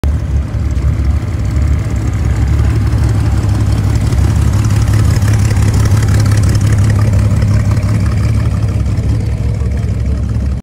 Sweet Ford T- Bucket
January 2024 car show  at Havasu 95 Speedway!  Great turn out some awesome cars.    Hit the Classic Cars - Trucks - Hot Rods playlist button to view all my car videos.  CHASING HAVASU